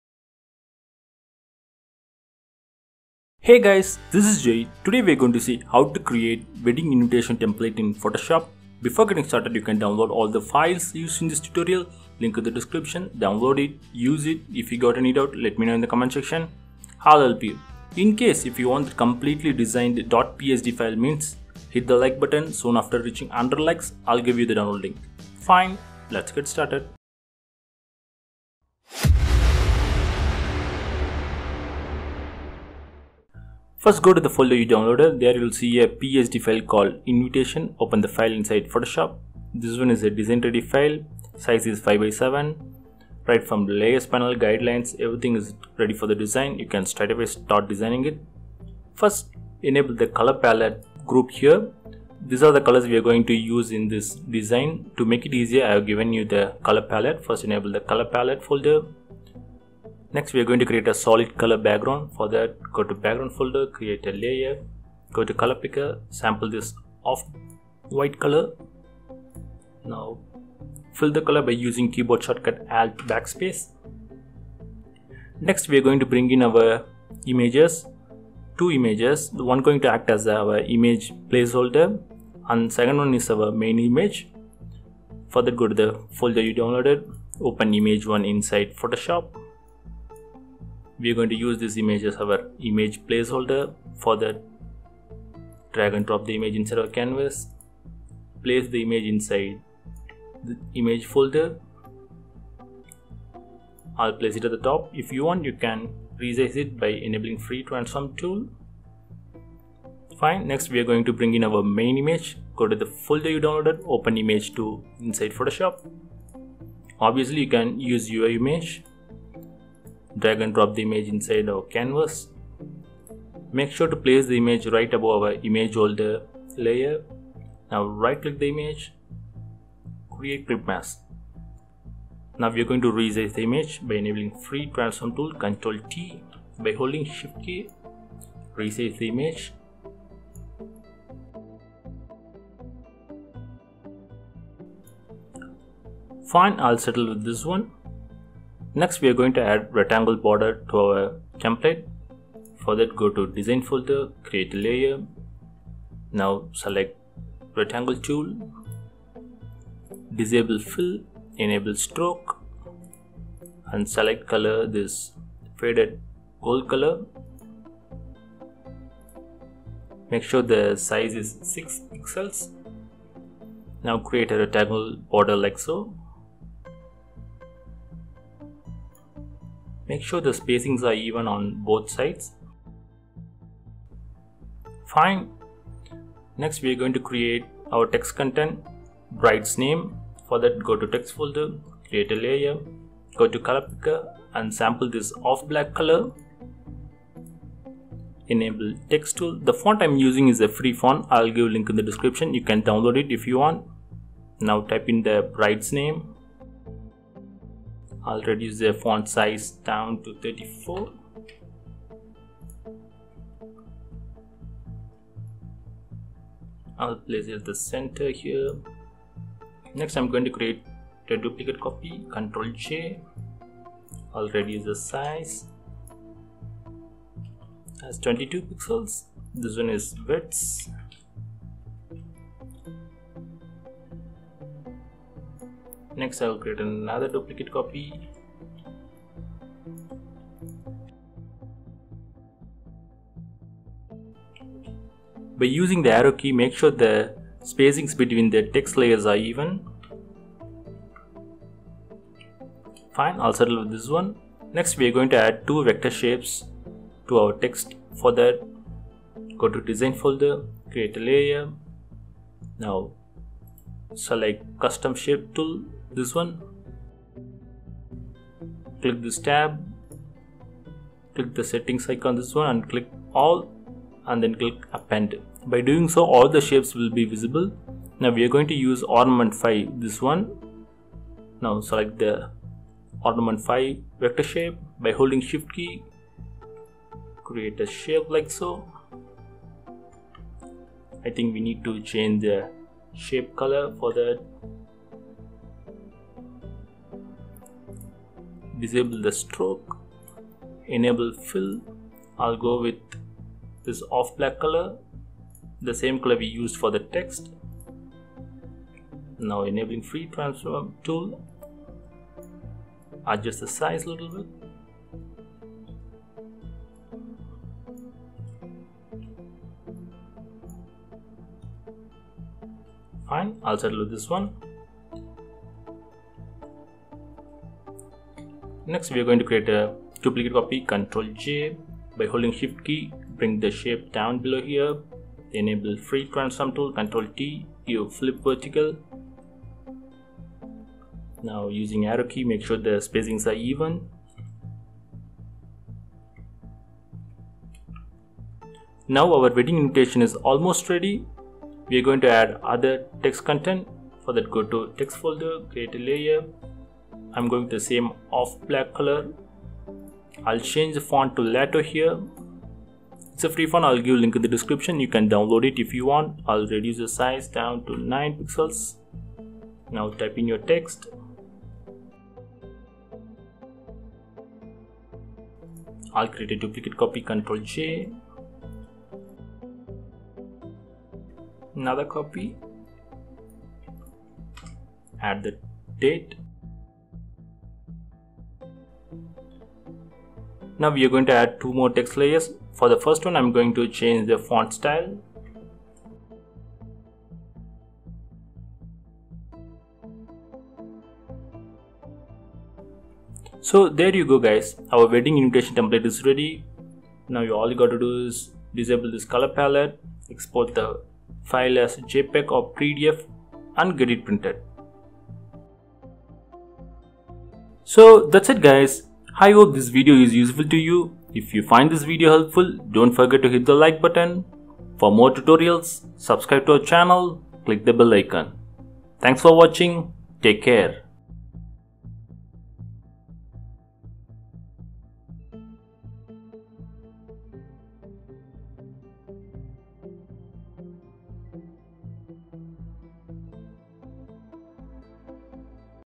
How to Create a Wedding Invitation Card in Photoshop | PE37
In this tutorial we are going to learn how to create attractive wedding invitation card in photoshop.

Fully Designed . PSD File INCLUDED
DONE